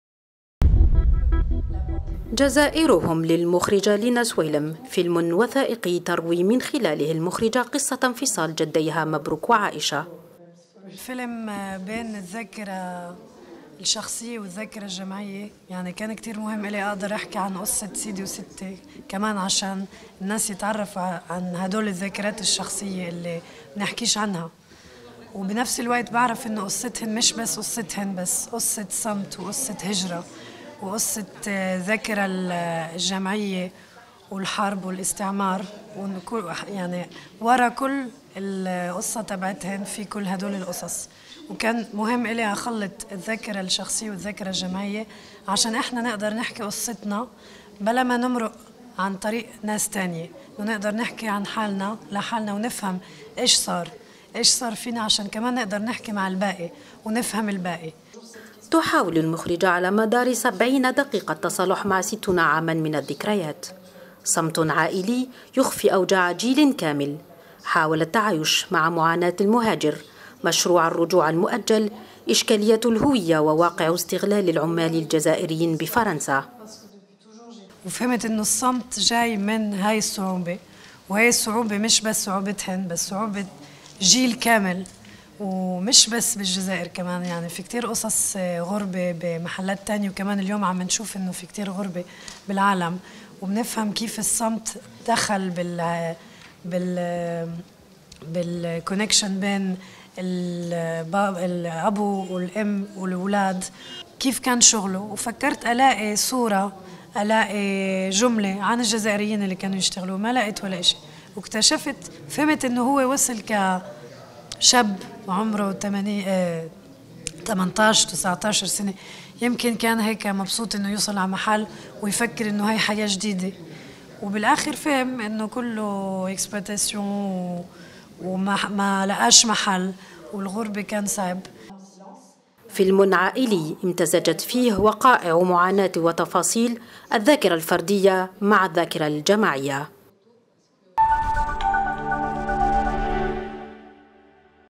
سينما: عرض الفيلم "جزائرهم" أمام الصحافة
الجزائر (وأج)- عرض امام الصحافة بقاعة سينماتك، الفيلم الوثائقي "جزائرهم" للمخرجة لينا سوالم جوا، الذي يعالج مسالة الوطن و الذاكرة و يلخص معاناة المبعدين عن اوطانهم و مجتمعاتهم بطرق او باخرى،على ان يعرض هذا الوثائقي ابتداء من الجمعة 10 ديسمبر و الى غاية الـ 15 منه في قاعة سينماتك و بالمعهد الثقافي الفرنسي.